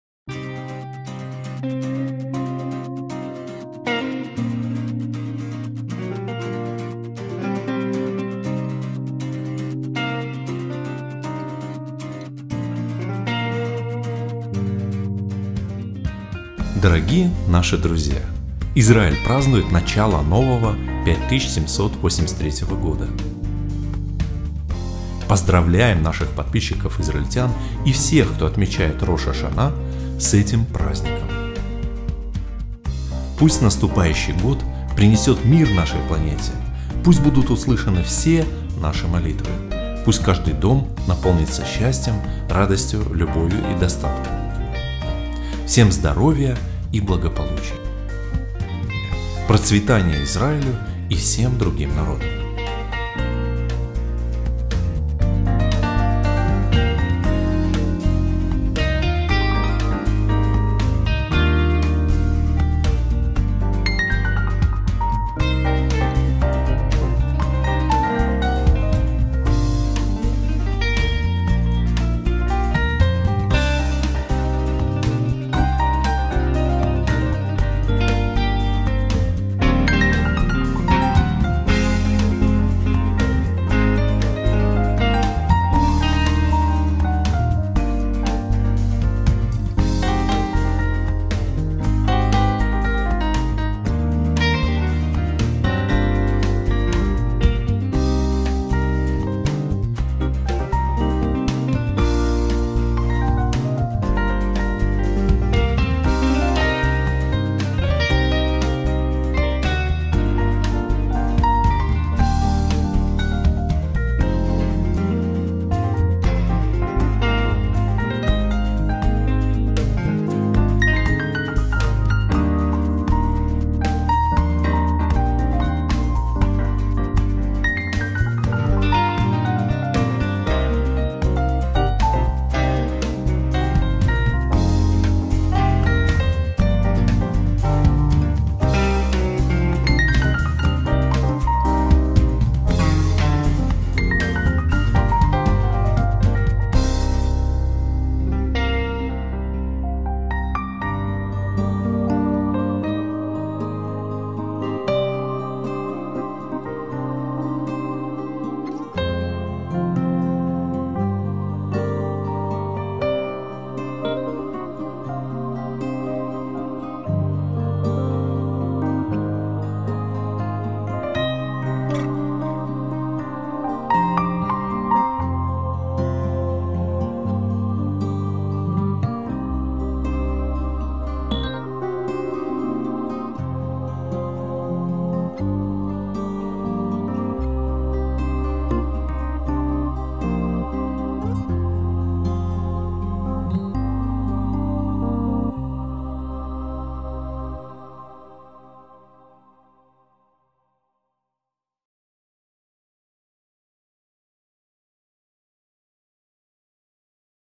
С Новым годом, Израиль!
Дорогие наши друзья, Израиль празднует начало нового 5783 года. Пусть наступающий год принесет мир нашей планете, пусть будут услышаны все наши молитвы, пусть каждый дом наполнится счастьем,  радостью, любовью и достатком.  Всем здоровья и благополучия. Процветания Израилю и всем другим народам.

Видео, музыка и исполнение
Павел и Лариса Платоновы
Арт-группа Запад и Восток
Иерусалим